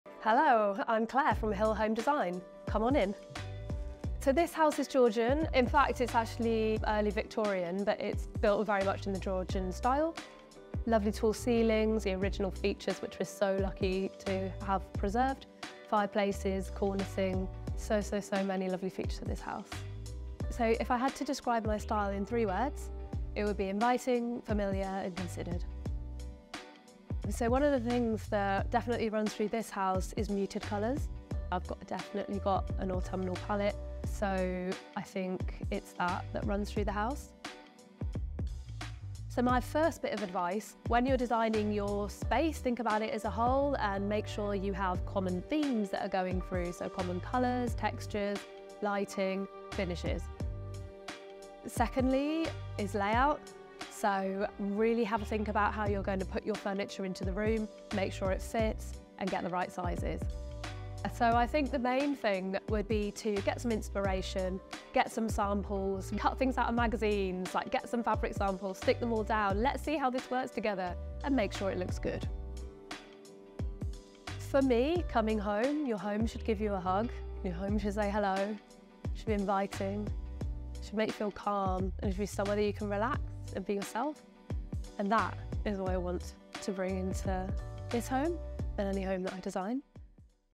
English Georgian Interiors - Claire Hill
Sensitive treatment of the original features and purposeful exaggeration of its Georgian / Late Victorian proportions have been at the core of Claire Hill's approach to interiors.

Interior design is often seen as an external force imposed, but when examples like this home come along - you can really tell when a design is manifested through living in a space.

We adore the tonality of Claire's designs, in its constraint, but also its confidence.